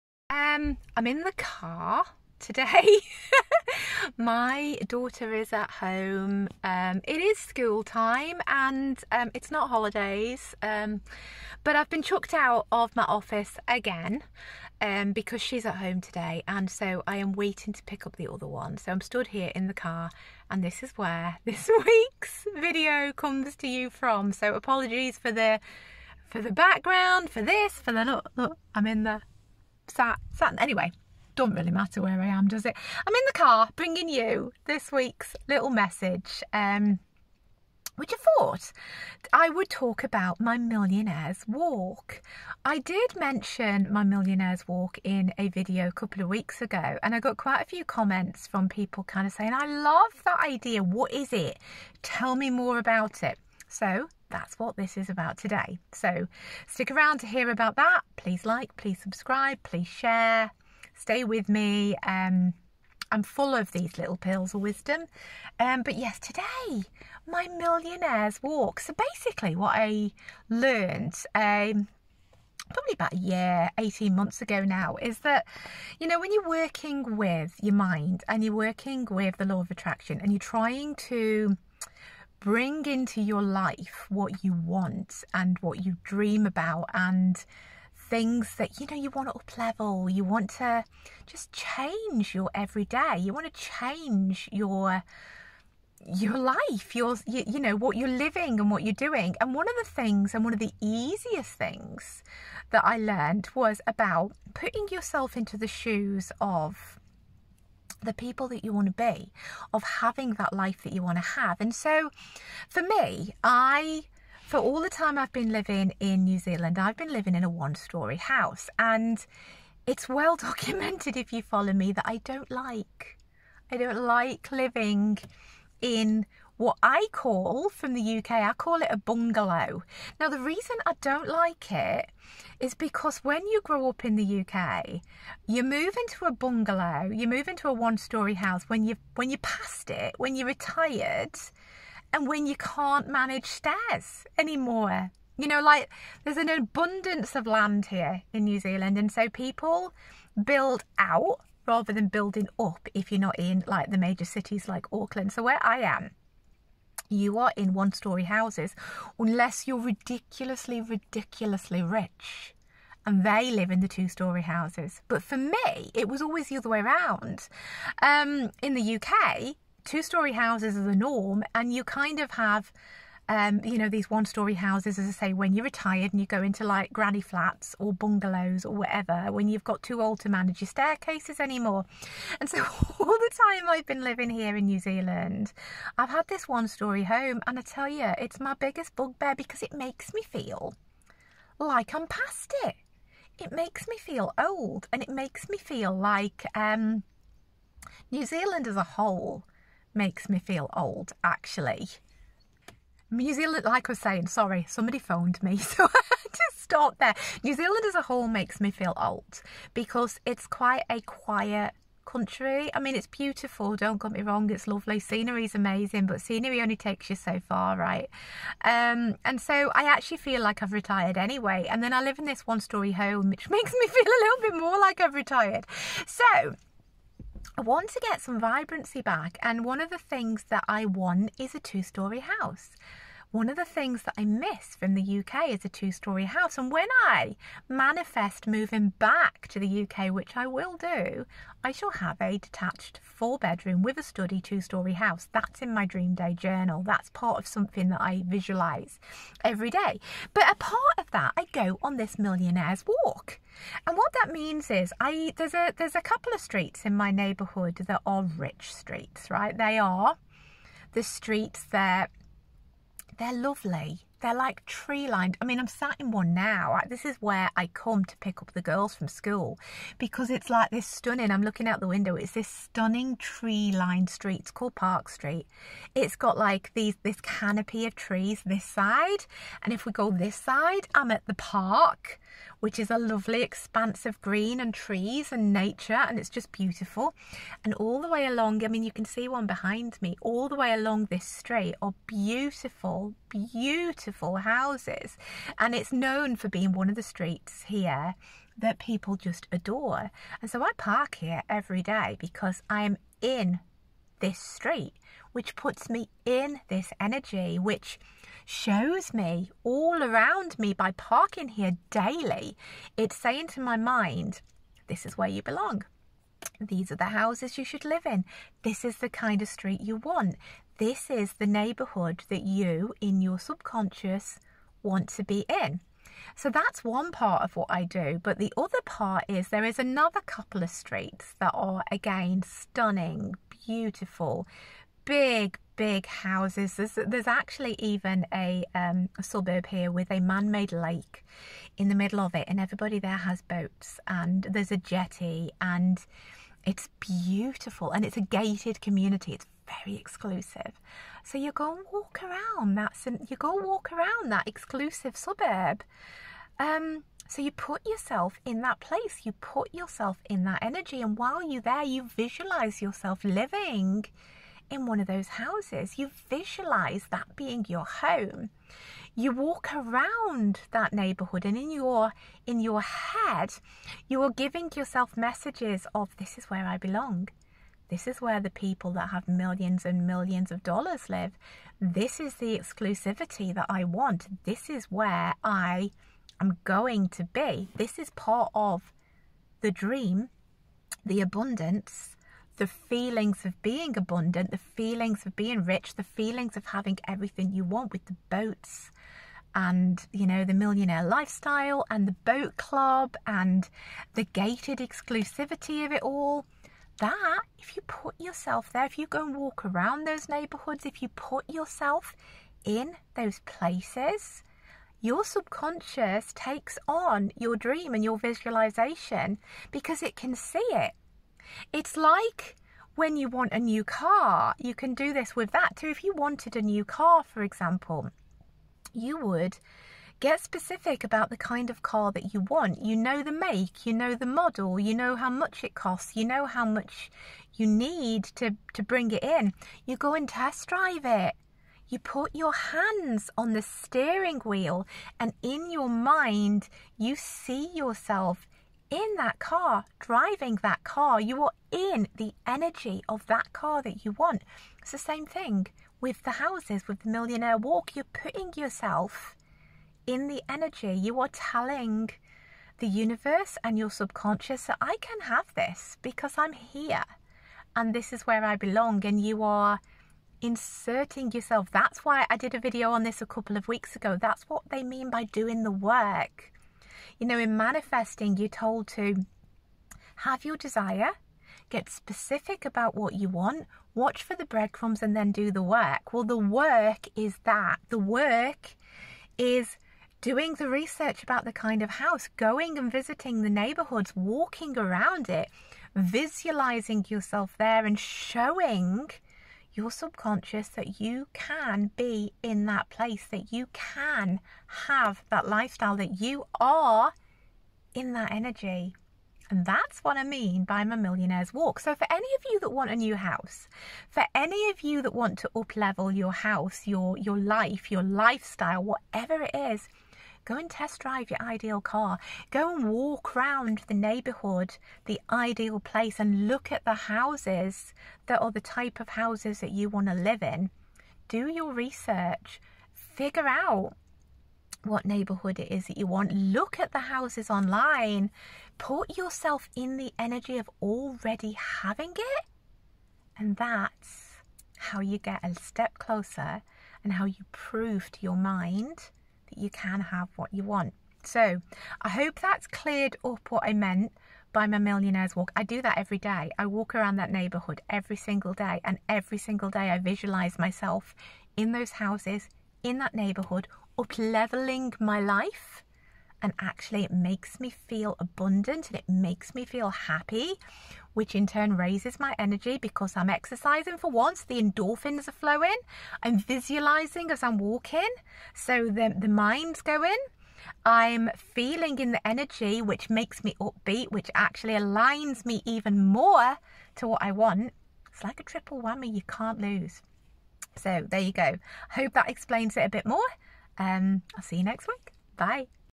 My Easy Manifesting Habit
Today I share more on my easy to do and implement manifesting technique, its so easy literally anyone can do it

“You can’t just visualize then go eat a sandwich” is a famous quote from Jim Carrey when talking about how he became a millionaire and basically crafted the life of his dreams.

In this video I am going to expand on the idea of my taking a millionaires walk each day to work alongside my visualisation and dream day process which I talk about in other videos.

 It goes without saying that the law of attraction does require some action.

Visualisation and positive thinking are crucial when it comes to using the law of attraction, but you have to meet the universe halfway with the steps you take as well. That's where my millionaires walk comes in, actually putting myself in the environment of the places where I want to live, in the feelings of where I want to live, in the energy of the places I want to live literally triples and super hypes my manifesting - and the best bit is its so easy.

Or you can join my facebook group (just copy and paste the link)

My Book, Your Life by Design

xx